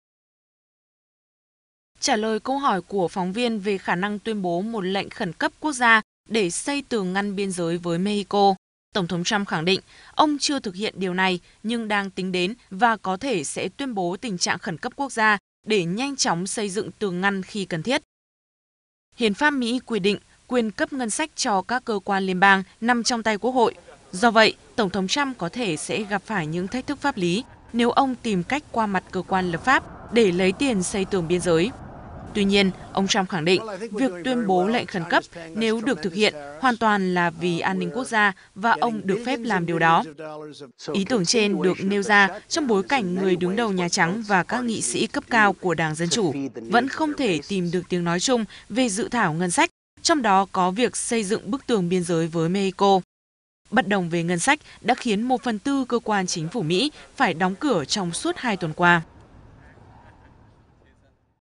Tổng thống Mỹ muốn dùng quyền khẩn cấp để xây tường biên giới
Đây là lần đầu tiên Tổng thống Trump đe dọa "qua mặt" các nghị sỹ trong bối cảnh đảng Dân chủ vừa tiếp quản Hạ viện.